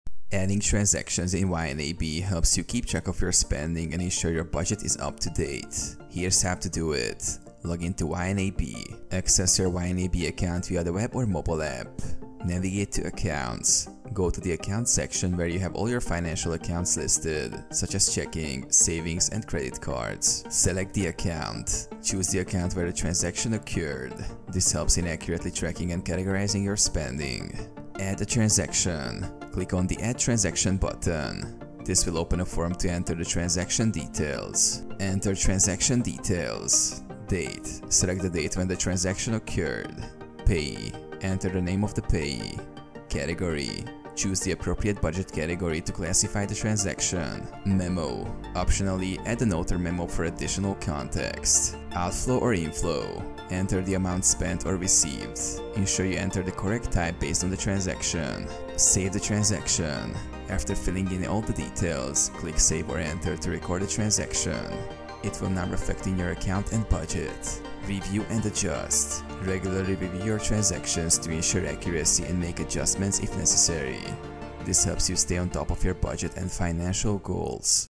How To Add Transactions In YNAB? (2024)
In this video I will explain How You Can Add Transactions In YNAB In 2024.